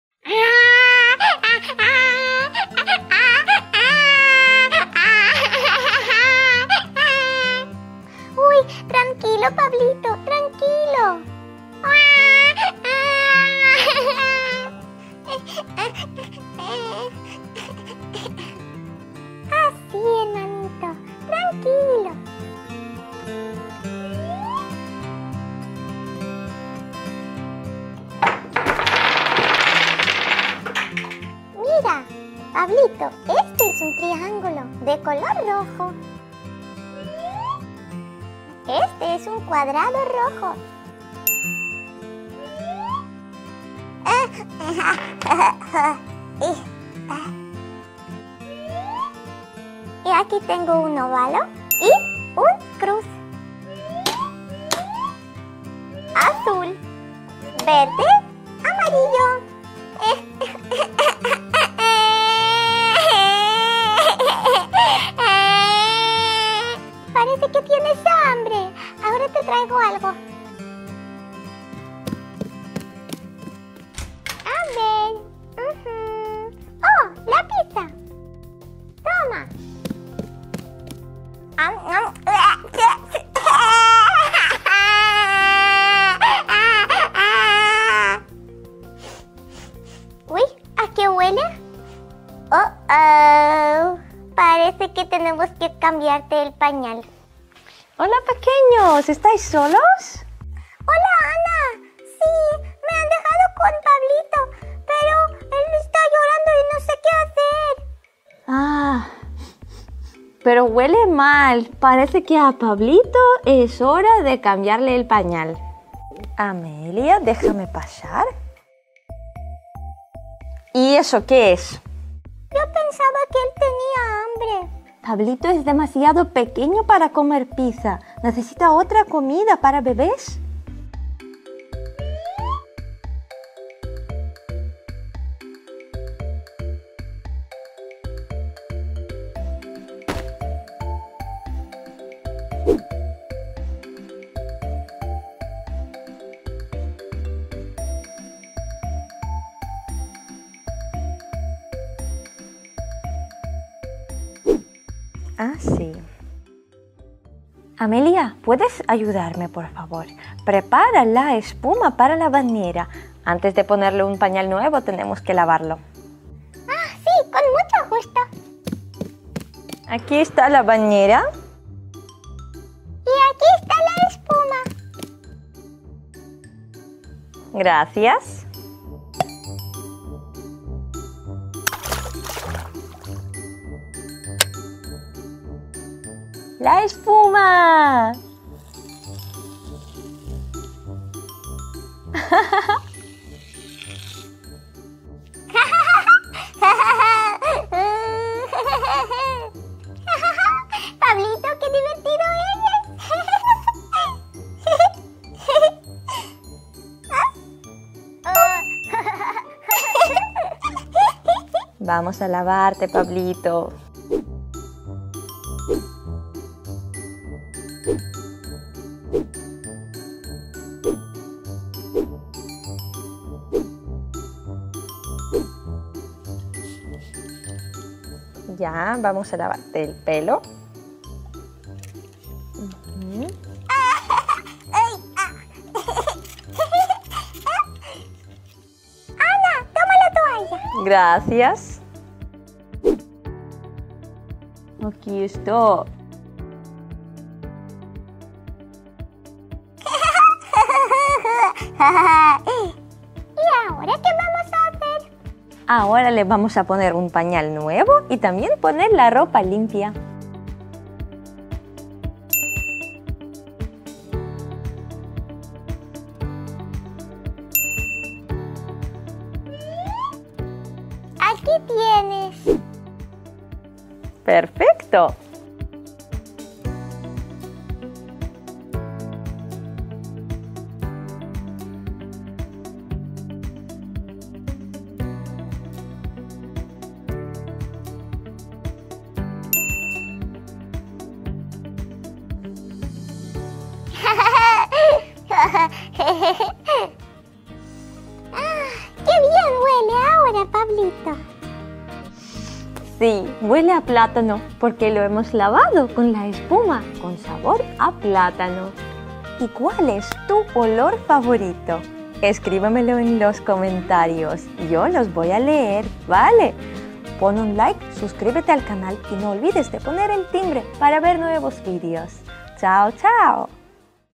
Cuidamos al bebé Pablito. Vídeos de muñecas Baby Born. Vídeos para niñas
El canal Vídeos para niñas presenta nuevo vídeo con Ana y las muñecas Baby Born.

El bebé Pablito está llorando por eso su hermana decide jugar con él para distraerlo un poco. Pero su hermanito no para de llorar. Parece que tiene hambre... o quiere que le cambien el pañal. Ha venido Ana para ayudarles. Vamos a preparar la bañera con espuma para Pablito. Mira, nuestro bebé ya no llora. Lo secaremos con una toalla muy suave, le pondremos un pañal nuevo y la ropita. ¡Así es! No ha sido tan difícil.

👶¿Qué está haciendo pequeña Bianca?